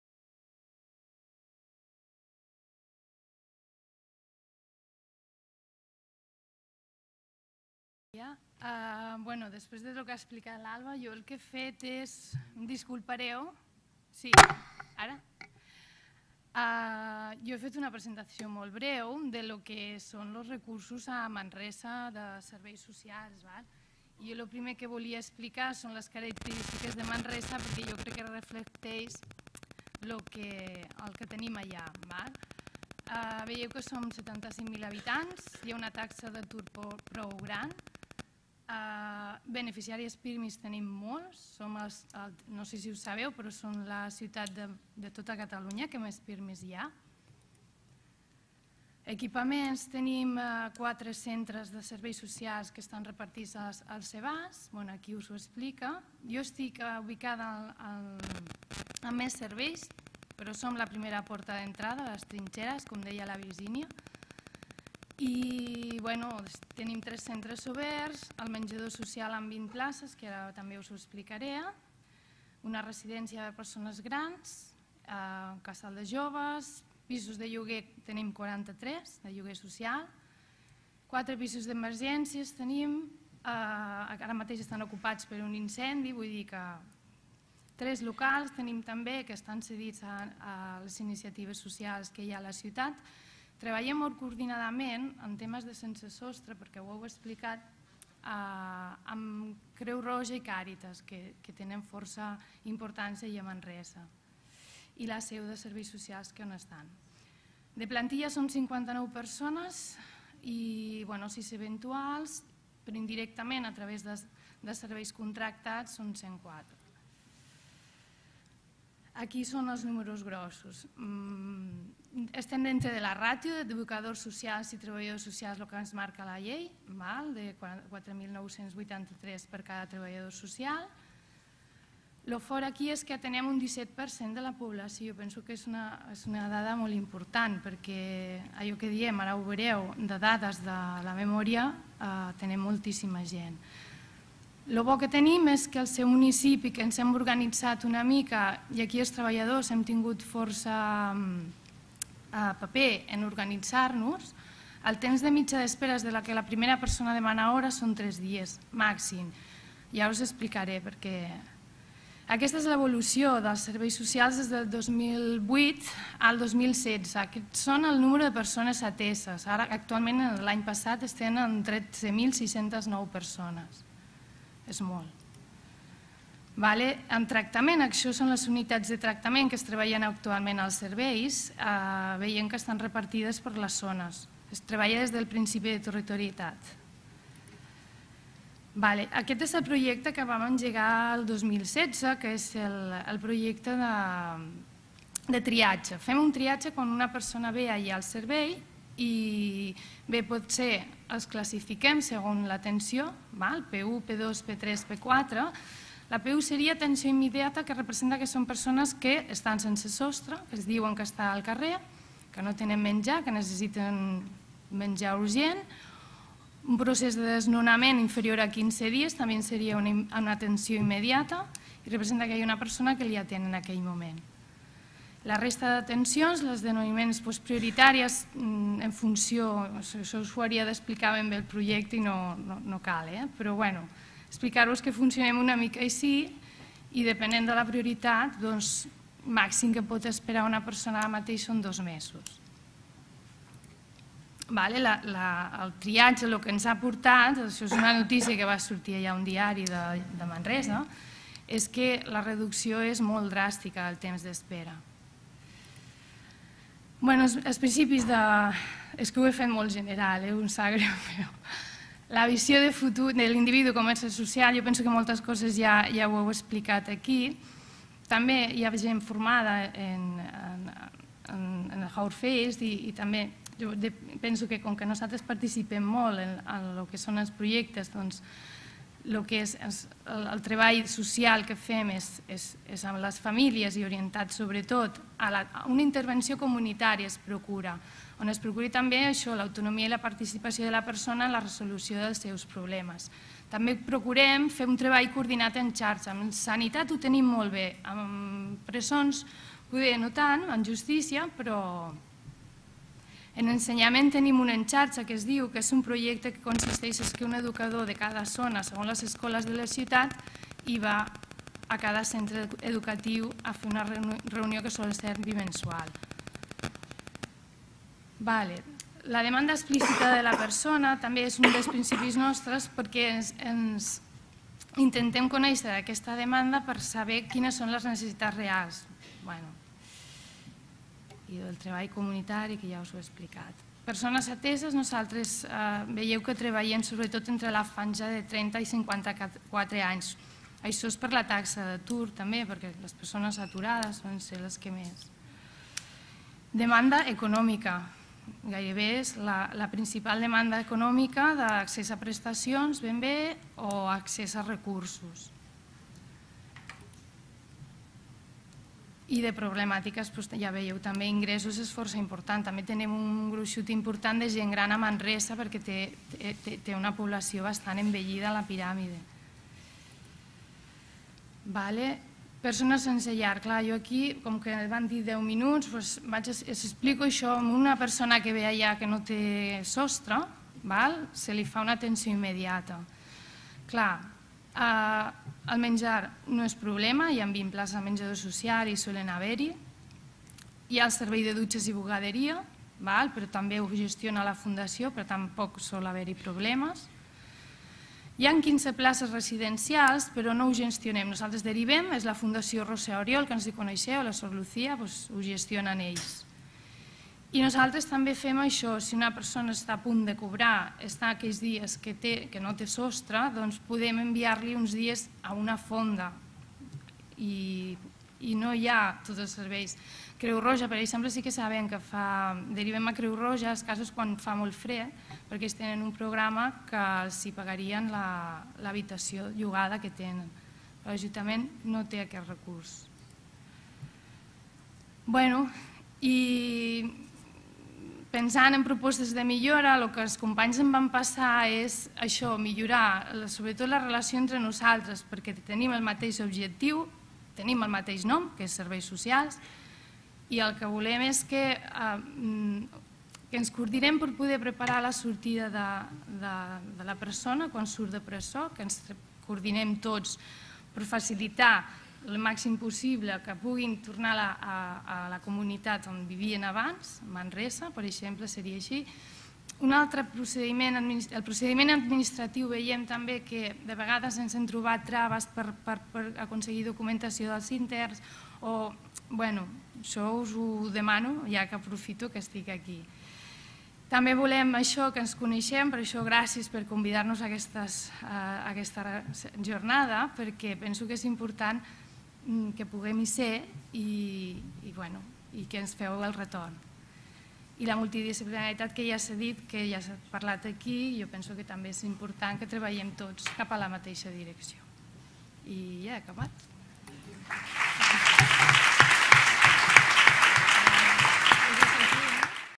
170927 XII Jornada de Treballadors Socials del Departament de Justicia
Sensellarisme: nova mirada davant les necessitats d’acollida
Taula rodona - Pilar Descalzo López
Persones vulnerables i serveis socials